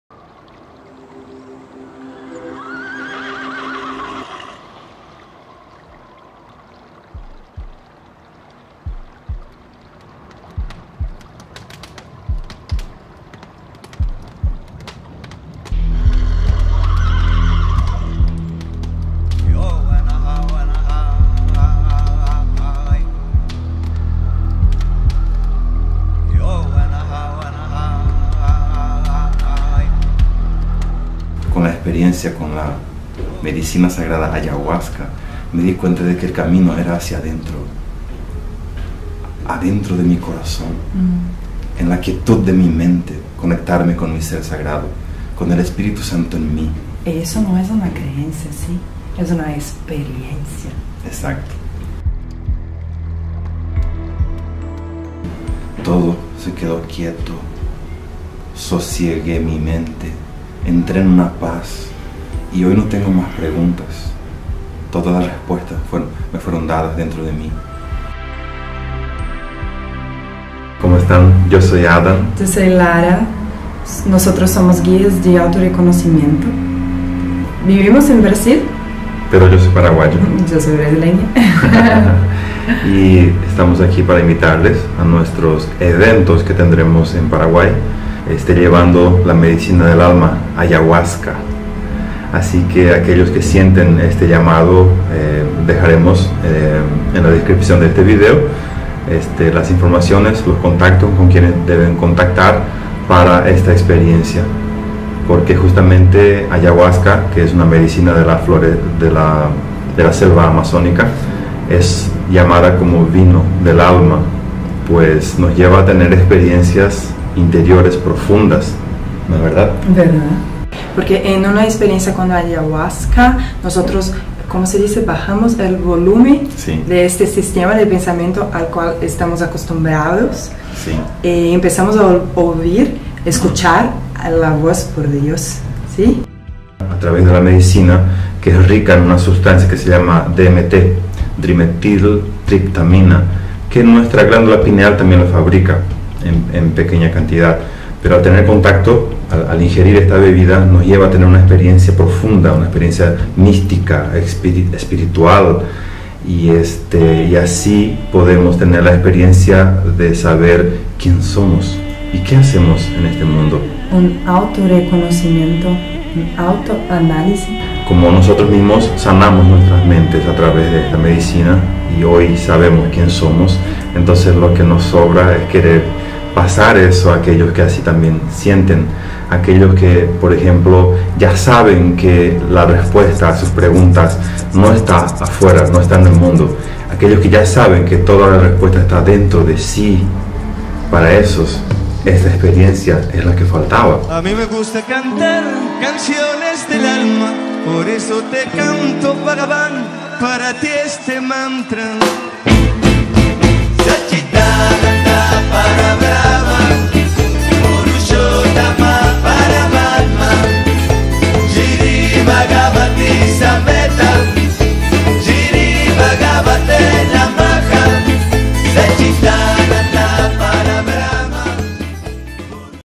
AYAHUASCA: Una Inteligencia Espiritual
PSICOTERAPIA ESPIRITUAL ANCESTRAL

AYAHUASCA es la Medicina Sagrada más reveladora, mas estudiada y más hablada actualmente en el mundo.

Es la Molécula de Dios, la leche de Pachamama que ilumina la mente de los buscadores cansados y de pura intención que quieren encontrar el sentido a Sus vidas. Sobretodo, los que están buscando la verdad sobre si mismos y que ya saben que la respuesta no está afuera, en el mundo, y si dentro de Sí mismo y están listos para aquietarse y reencontrarse.

AYAHUASCA és una Inteligência Espiritual, comprobada como:
Poderoso Antidepresivo, Estimulador de sinapses neurales y expansor de la conciencia.
Regulador de la ansiedad y del humor.
También muy utilizado en el Brasil y con muchísimo éxito en el tratamiento y cura de los dependientes de medicamentos y demás drogas. Así como tantos otros beneficios físico-mentales, además de la liberación de los males del Alma.
Y lo que nunca nos deja de sorprender es que todo esto lo encontramos en un retiro espiritual, donde consagramos con un simple té natural de los InDios de la selva Amazónica.
Y pedimos a Dios que se haga Su Voluntad así
en la tierra como en el Cielo.

Quieres venir con nosotros?

Adan y Lara
(Guias de AutoReconhecimento)